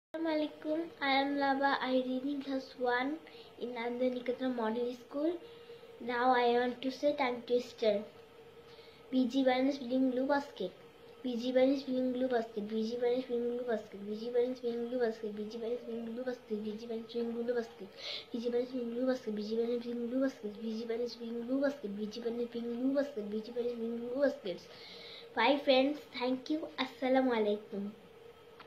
Kids Spoken, Topic : Tongue twister by Naba, Class 1, School :
Liakat's Kids Spoken English Course. It's a weekly presentation which is mandatory for every student of our school.

Student's Name : Naba
Class                    : One
School                 :

Hotline :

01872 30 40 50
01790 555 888

Lalmatia, Mirpur 10 & Uttara.